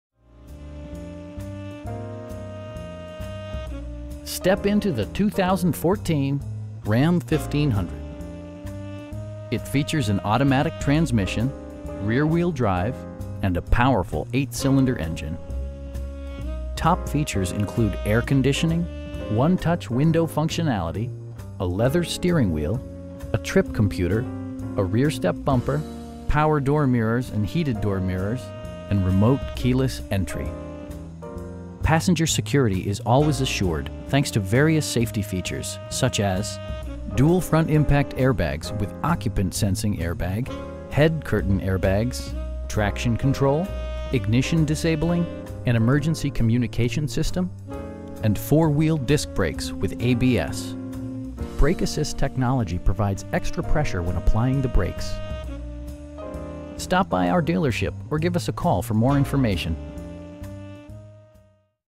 Hemi 2014 Ram 1500 Austin TX | Mac Haik Ram Georgetown
2014 Ram 1500 available for sale in the greater Austin area at Mac Haik Dodge Chrysler Jeep Ram in Georgetown. Come find out what you have been missing and browse our entire inventory This gorgeous bright white 2014 Ram 1500 features the legendary Hemi engine and is one of the most dependable trucks on the road. Make the short drive from Austin and come visit us today!